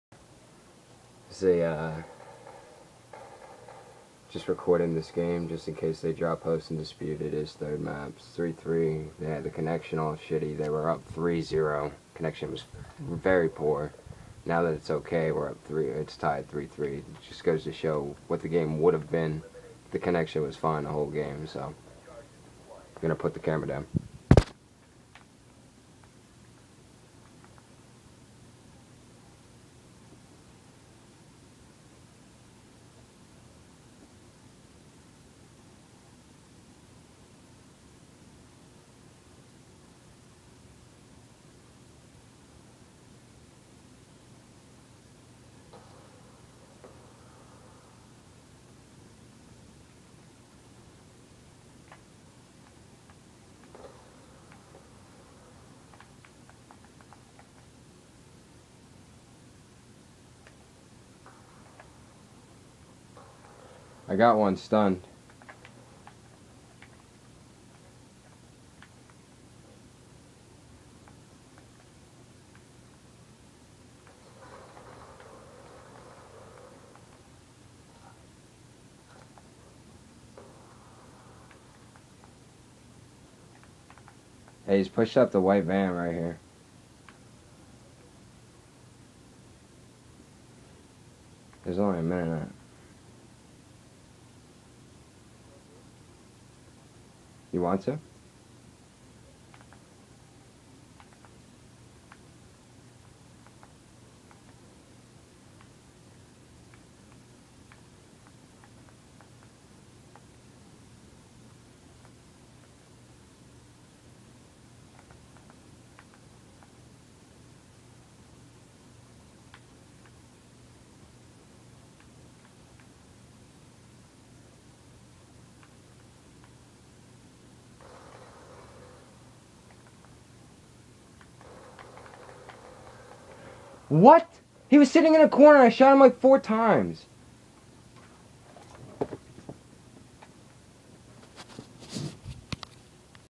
Proof Vs Simply Recked n Hercuhles. Proof we came back 0-3
Video ends @  25 seconds. i died at the end and lost faith and turned ipod off lol saint clutches the last round FTW and they dropped host. Their connection on this map was HORRIBLE, we were down 0-3 because of it... then all the sudden the connection is okay and we come back to win 4-3 they disputed